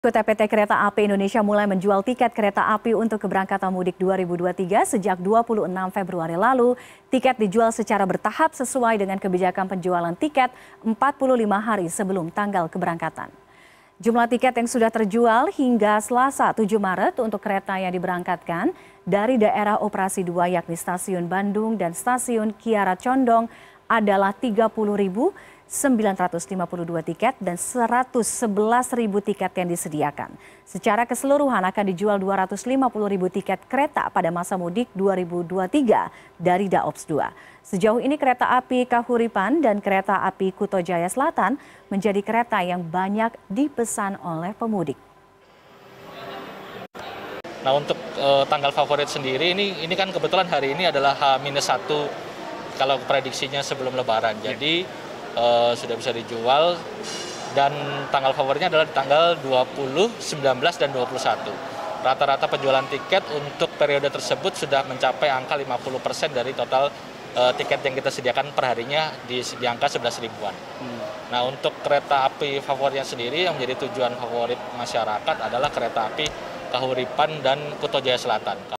Tiket Mudik KAI Sudah Mulai Dijual Bertahap
PT Kereta Api Indonesia mulai menjual tiket kereta api, untuk keberangkatan mudik 2023, sejak 26 februari lalu. Tiket dijual secara bertahap, sesuai dengan kebijakan penjualan tiket, 45 hari sebelum tanggal keberangkatan.